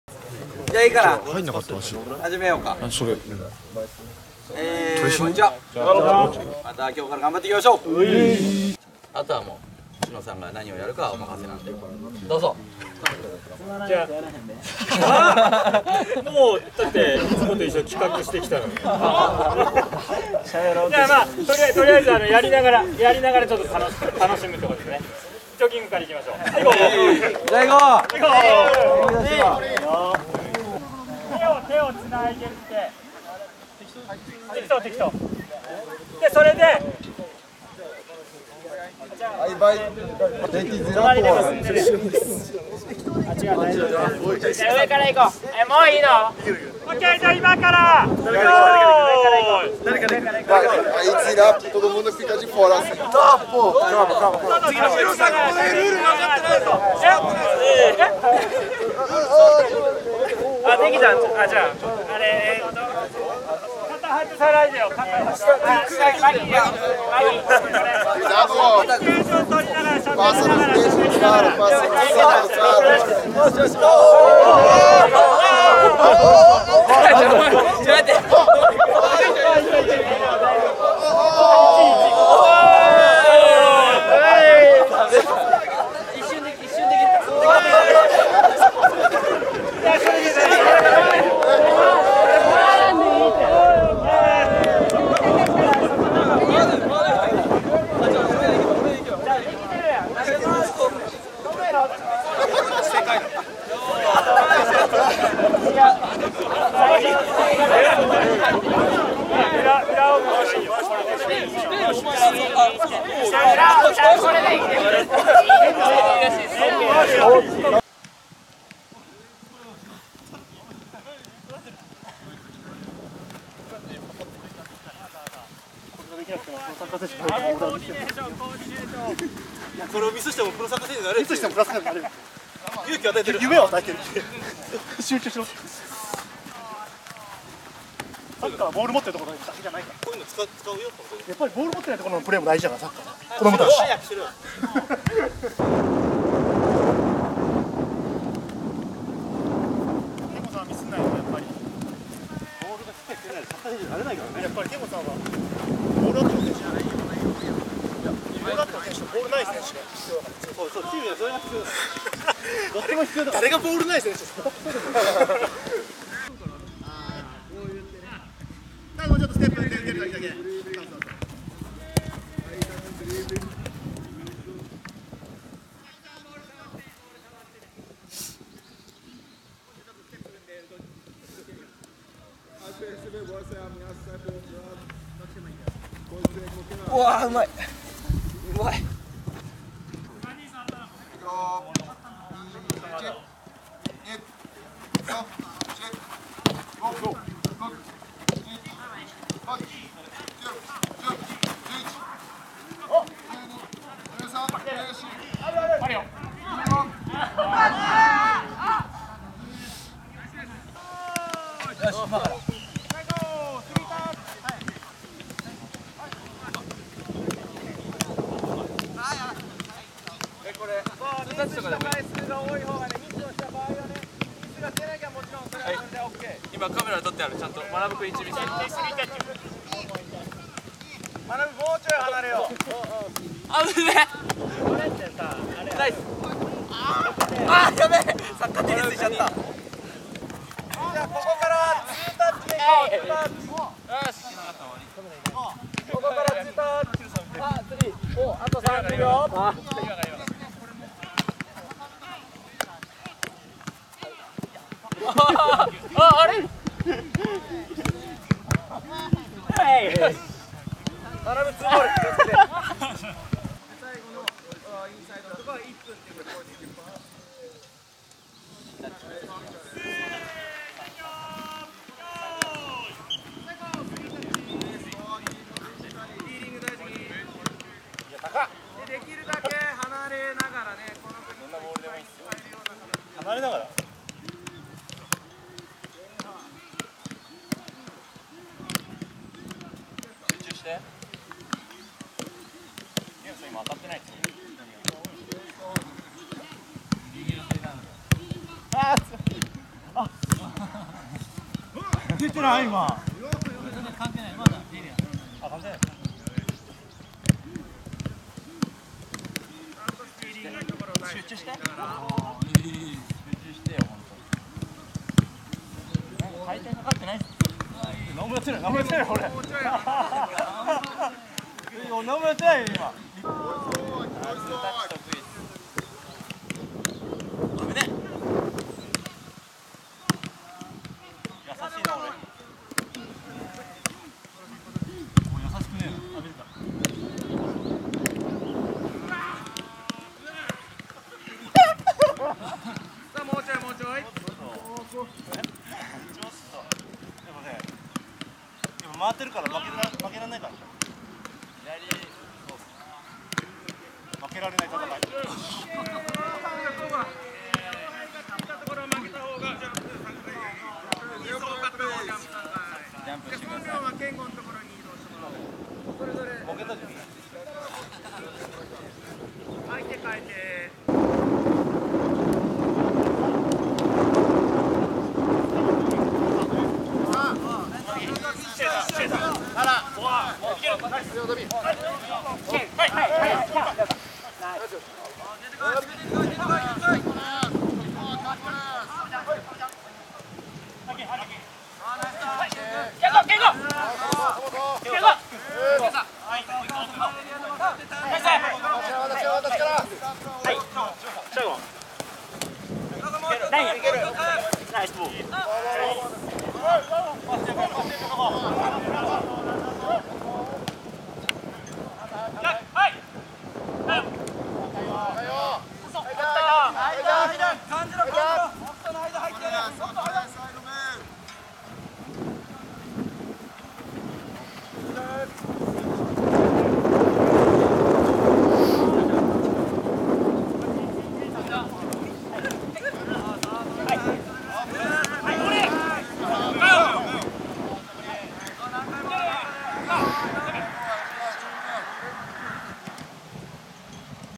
【公式】2019 宮崎県綾町キャンプ（前編）
2019年1月16日（水）～1月26日（土）の期間、宮崎県綾町で実施したキャンプの様子をお届けします。